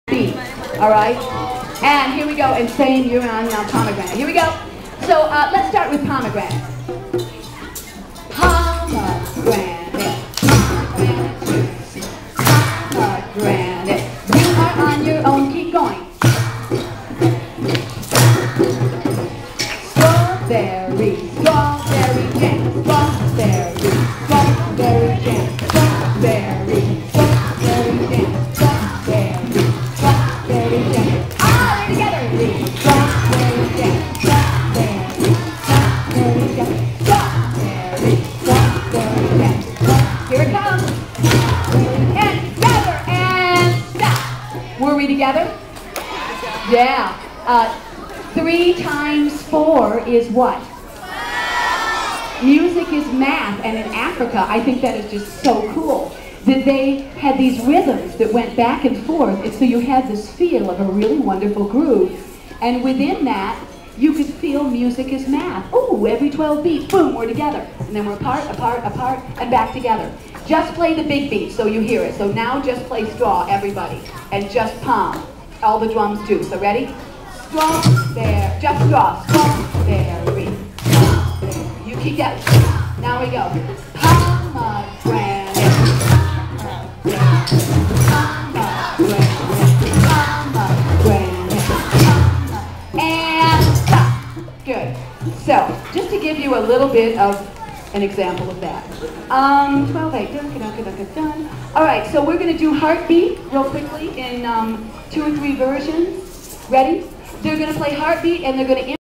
Mary Knysh Music Improvisation Workshops: Music is Math
In this workshop performance, Mary demonstrates a 12 beat rhythmic cycle that contains 3 sets of four beats and 4 sets of three beats by using the syllables of words. Students listen for the resulting BIG BEAT that occurs every 12 beats when the cycles meet! This can be a terrific way to explore fractions!